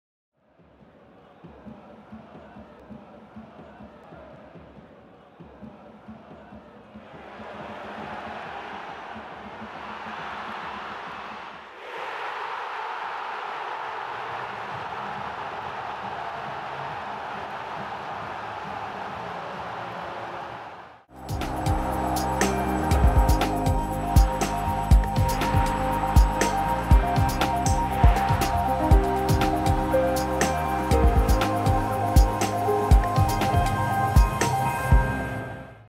Tomas Brolin v England, 1992: 60 Great UEFA Goals
England's 1992 UEFA European Championship campaign ended with a defeat at the hands of host nation Sweden in the group stage, thanks to wonderful one-touch passing and a finish to match from Thomas Brolin who, with eight minutes to go, secured a 2-1 win.

Råsundastadion, Solna
Euro '92
17/06/1992